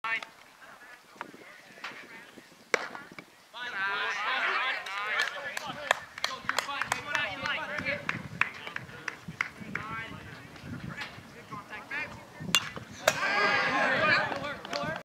Rob working behind the plate
Rob has shown alot of growth over his 4 years with Ridge. vs Frostproof 2/5/2016